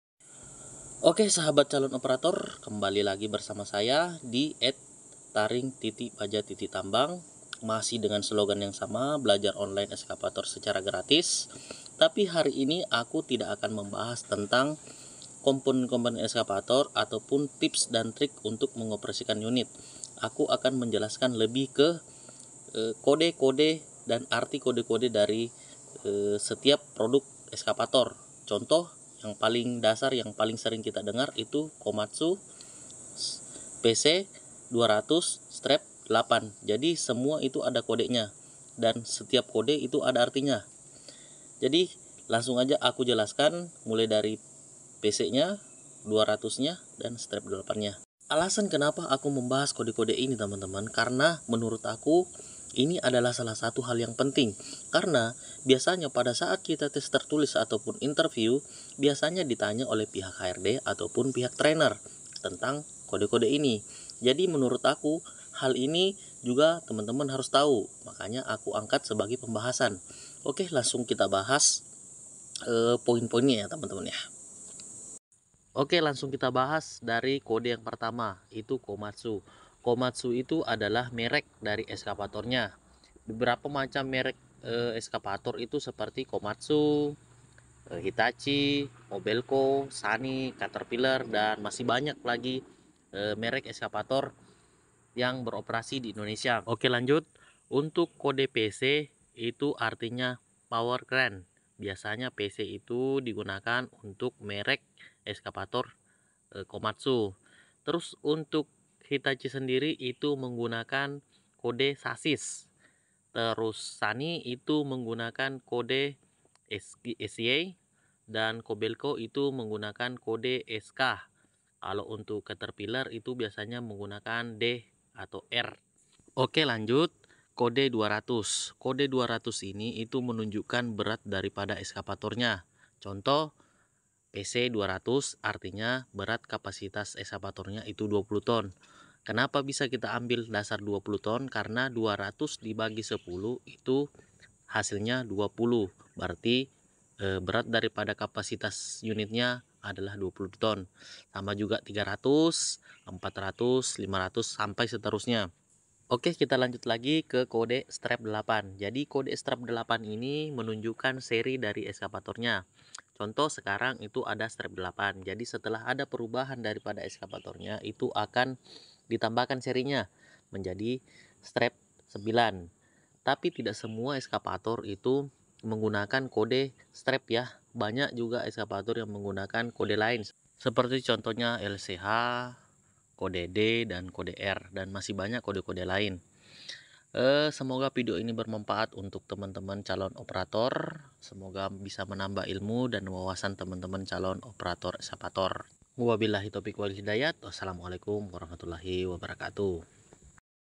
Arti kode pada excavator
arti kode pada excavator @Taring.baja.tambang

Begini cara membaca arti dari kode Excavator
#-8
sebagai calon operator excavator kita harus tau membaca kode ini karna ini sangat penting saat kita tes interview atau tes lapangan biasanya ditanya Oleh HRD atau trainer saat tes
jadi sebelum ditanya soal kode2 ini calon operator excavator harus tau
semagat calon operator